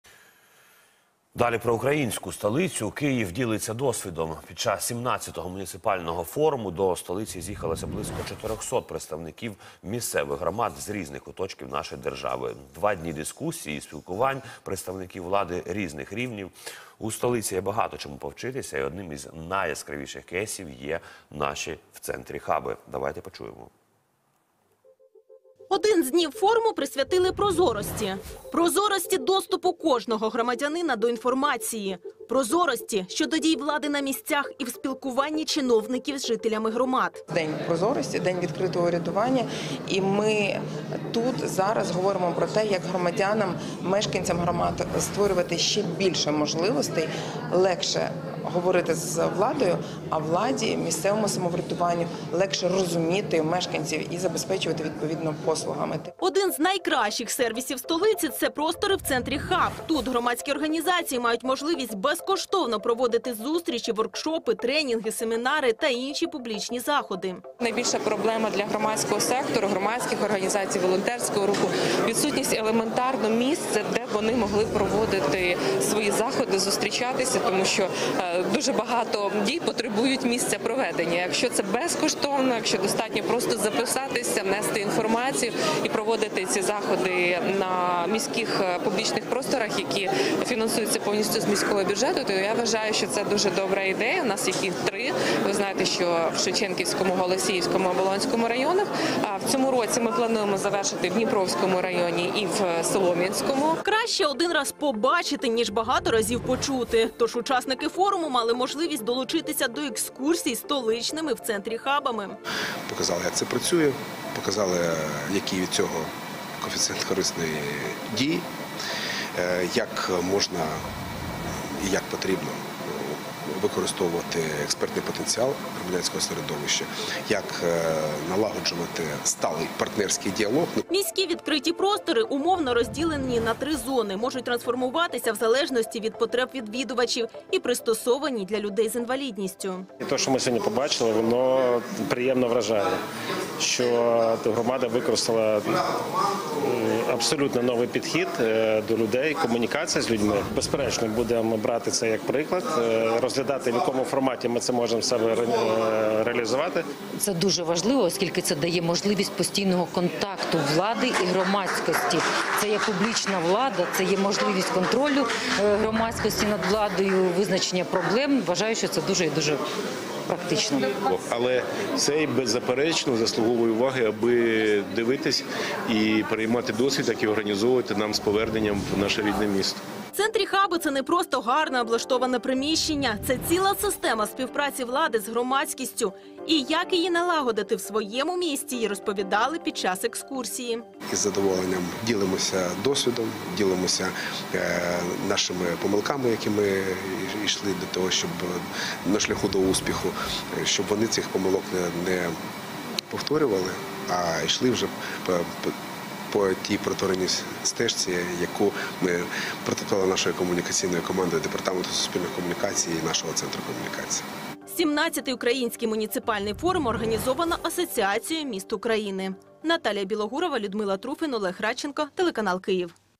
День Прозорості в рамках XVII Українського муніципального форуму
Участь у Дні Прозорості взяли представники близько 400 громад, органів державної влади, міжнародних інституцій, європейських муніципалітетів, дипломатичних представництв, ЗМІ.